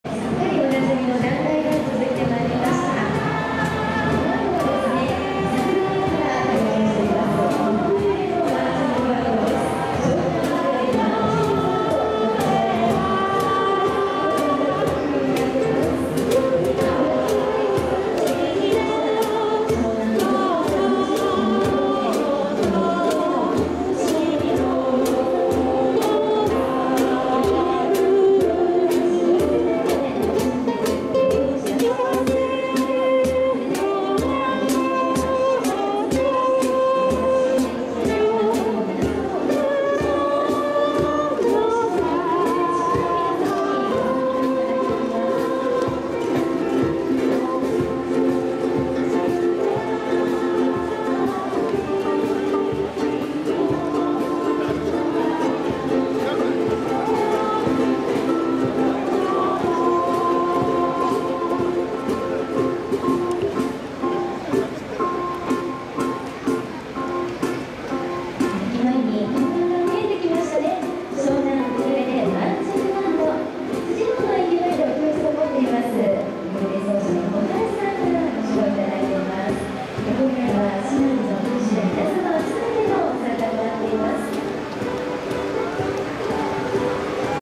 平成28年 第43回 藤沢市民まつり 大パレードです。
第４３回藤沢市民まつり　南口大パレード。
毎年、たくさんの来場者でにぎわう「藤沢市民まつり」は、
1974年（昭和49年）から開催が続いており、
市民のふれあいと個性豊かなふるさとづくりを推進するため、
市民自らが企画し、参加する市民文化の集大成の場として
藤沢市民にとっての伝統的な一大イベントのひとつとなっています。

大パレード、ステージイベント、屋台村などのイベントを中心に、
毎年9月の最終週の土曜日、日曜日の2日間、藤沢駅周辺会場を中心に、
市内各地で開催されています。